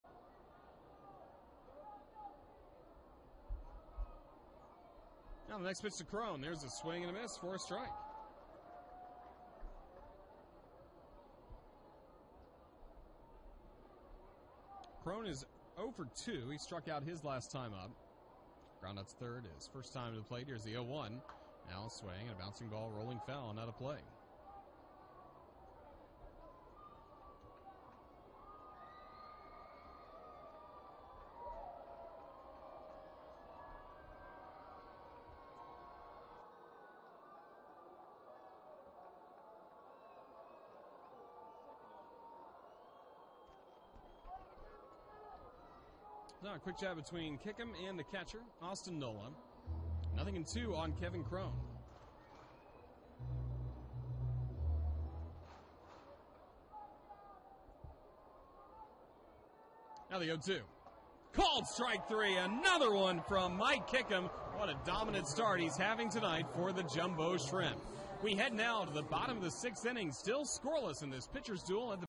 Jacksonville's Kickham equals career high in whiffs
5/27/17: Mike Kickham equals his career high with his 12th strikeout of the game for Jacksonville.